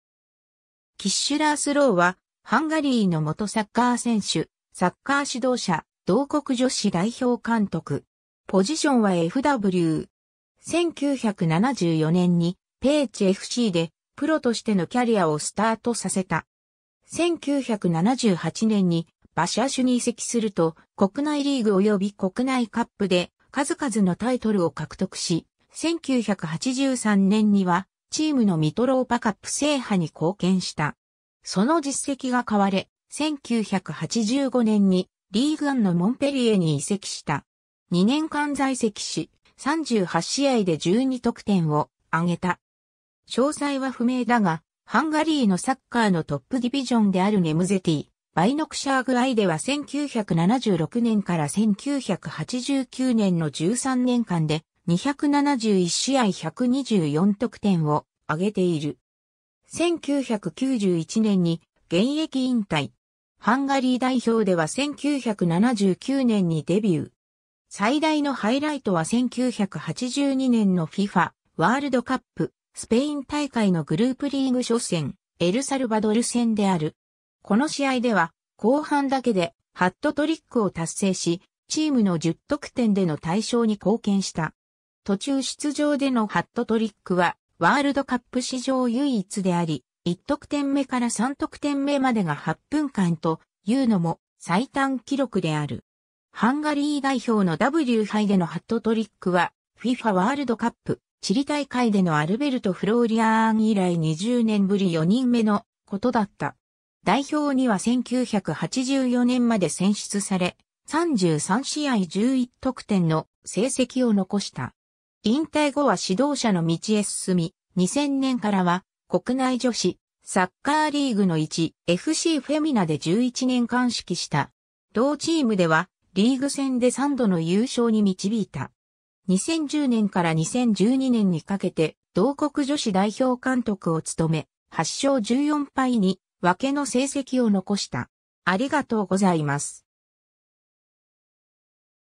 キッシュ・ラースロー
キッシュ・ラースロー（ハンガリー語：Kiss László 、1956年3月12日 - ）は、ハンガリーの元サッカー選手、サッカー指導者、同国女子代表監督。
 1974年に、ペーチFCでプロとしてのキャリアをスタートさせた。
1978年にヴァシャシュに移籍すると、国内リーグ及び国内カップで数々のタイトルを獲得し、1983年にはチームのミトローパ・カップ制覇に貢献した。
その実績が買われ、1985年にリーグ・アンのモンペリエに移籍した。
2年間在籍し、38試合で12得点を挙げた。
詳細は不明だが、ハンガリーのサッカーのトップディビジョンであるネムゼティ・バイノクシャーグIでは1976年～1989年の13年間で、271試合124得点を挙げている。
1991年に現役引退。
 ハンガリー代表では1979年にデビュー。
最大のハイライトは1982年のFIFAワールドカップ・スペイン大会のグループリーグ初戦・エルサルバドル戦である。
この試合では、後半だけでハットトリックを達成し、チームの10得点での大勝に貢献した。
途中出場でのハットトリックはワールドカップ史上唯一であり、1得点目から3得点目までが8分間(後半24分～後半31分)というのも最短記録である。
ハンガリー代表のW杯でのハットトリックは、FIFAワールドカップ・チリ大会でのアルベルト・フローリアーン以来20年ぶり4人目のことだった。
代表には1984年まで選出され、33試合11得点の成績を残した。
 引退後は指導者の道へ進み、2000年からは国内女子サッカーリーグの1.FCフェミナで11年間指揮した。
同チームではリーグ戦で3度の優勝に導いた。
2010年から2012年にかけて同国女子代表監督を務め、8勝14敗2分けの成績を残した。